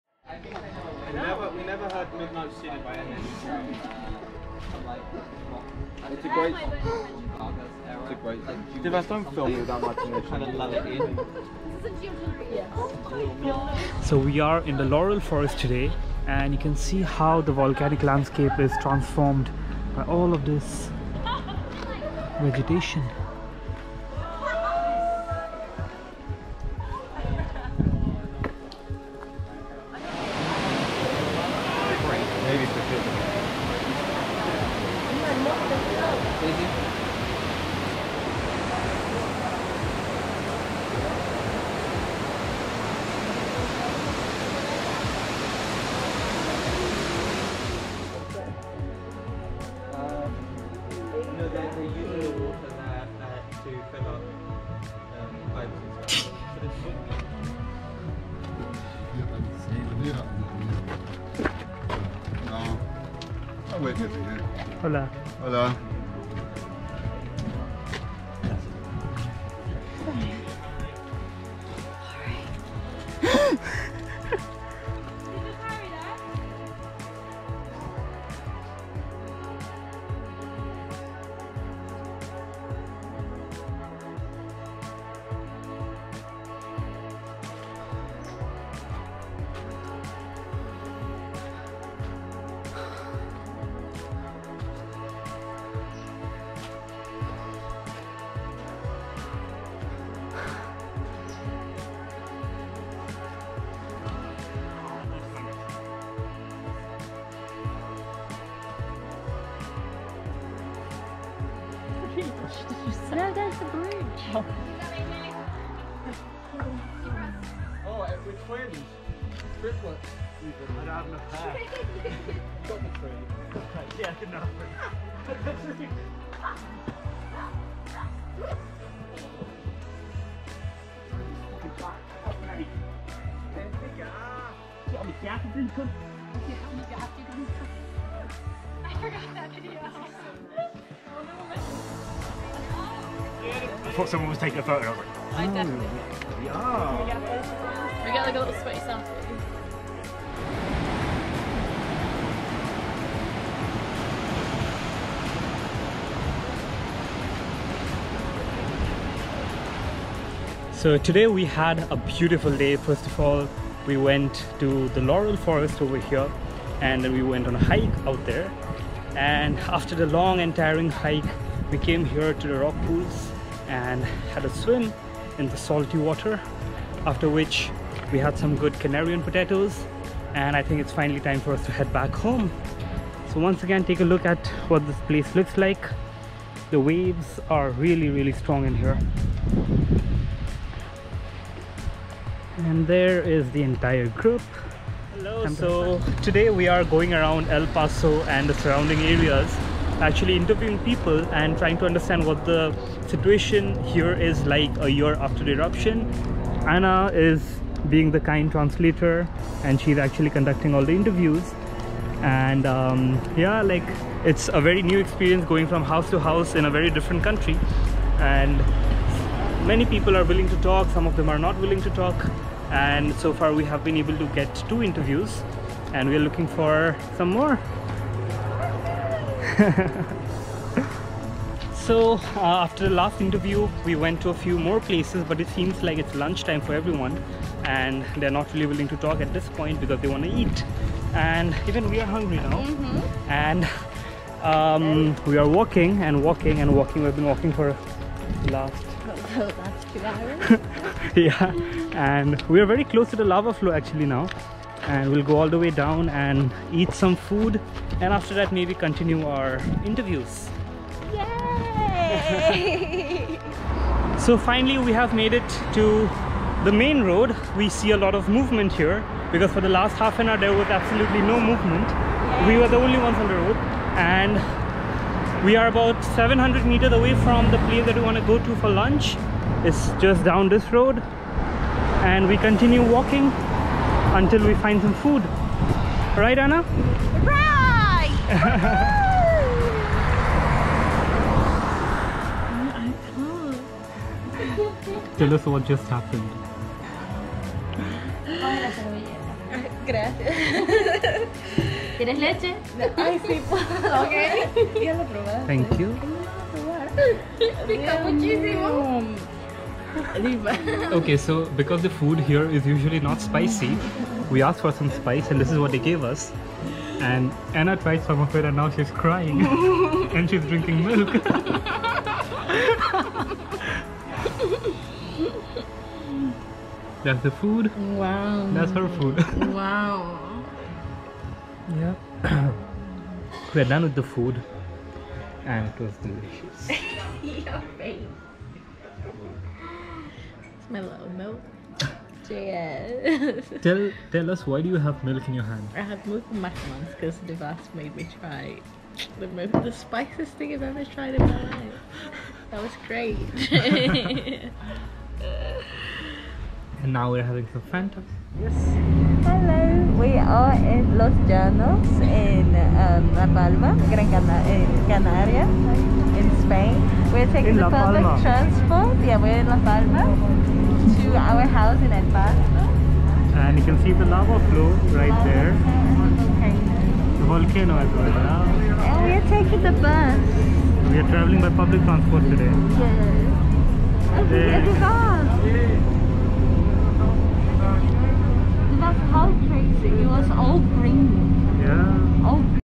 A Hike in the Laurel Forests of La Palma | Rock Pools by the Sea | Interviewing Locals
In this video, we go on a weekend getaway to the Laurel forests of La Palma. Laurel forests are a type of subtropical forest found in areas with high humidity and relatively stable, mild temperatures. The forest is characterized by broadleaf tree species with evergreen, glossy and elongated leaves. After a tiring, yet rewarding hike, we drive to the natural rock pools by the ocean, the most ideal way to cool off in a remote island paradise on a weekend.

In the following days, I go around the El Paso town with a dear friend Ana for some science communication activities with the locals. We interview locals about their experience dealing with the 2021 volcanic eruption on the island and the ground situation one year from the eruption. This happens to be the last video from La Palma for this tour.